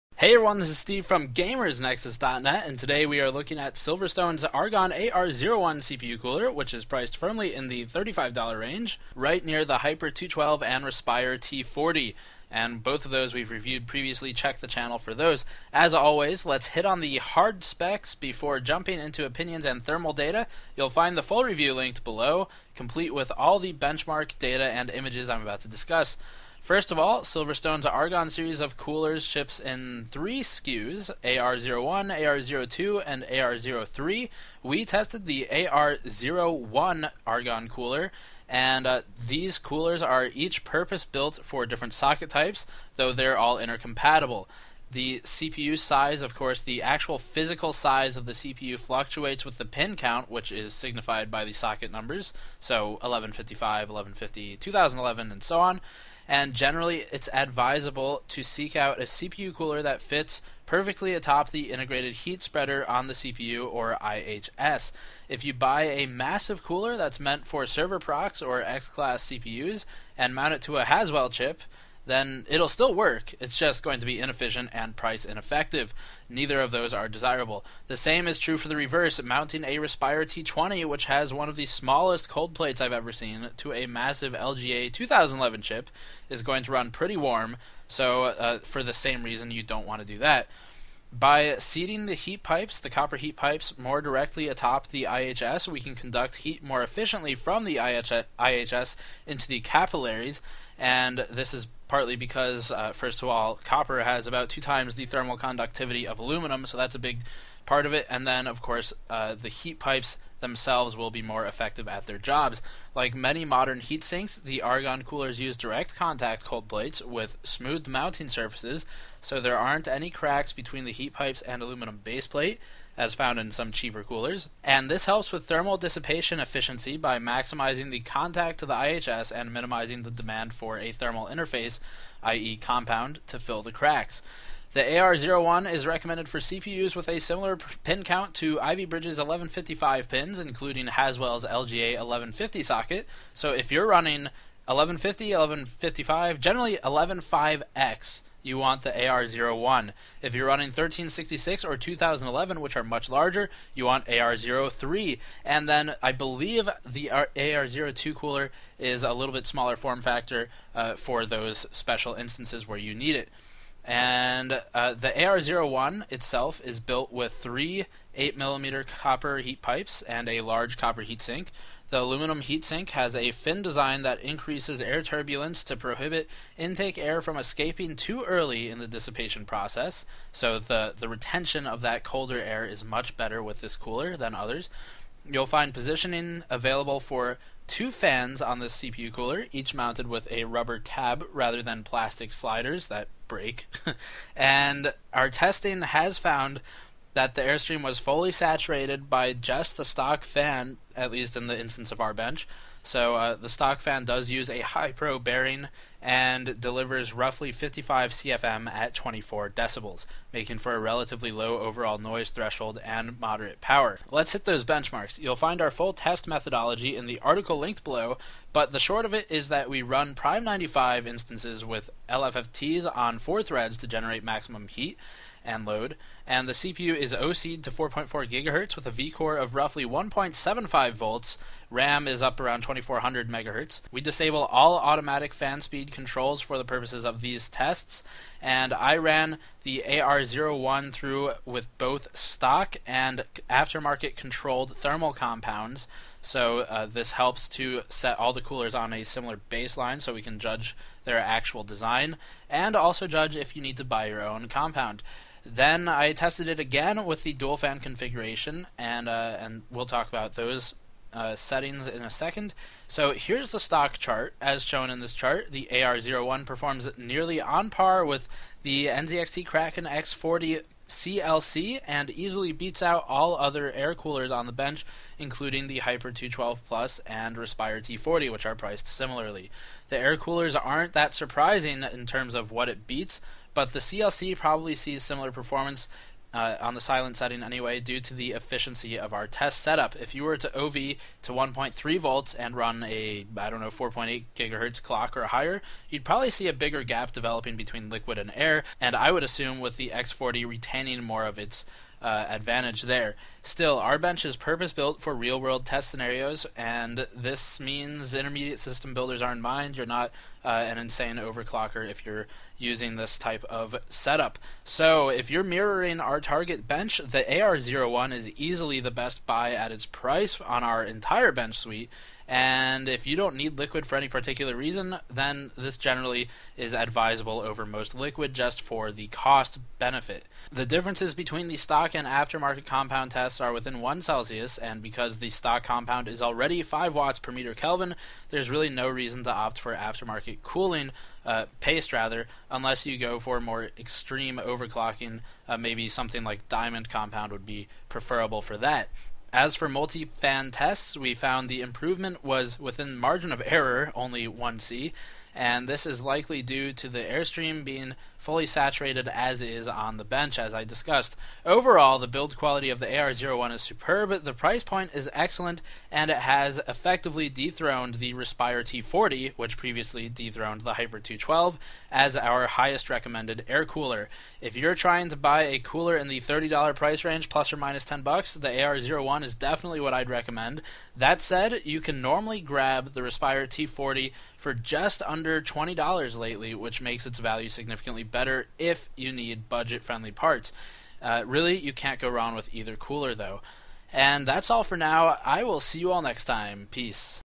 SilverStone Argon AR01 CPU Cooler Benchmark/Review vs. Hyper 212, T40, etc.
In this benchmark and review of SilverStone's Argon AR01 cooler, we pit it against the Hyper 212 Plus, Respire T40, and other air/liquid coolers for your PC build. Installation is shown for the AR01 throughout the video.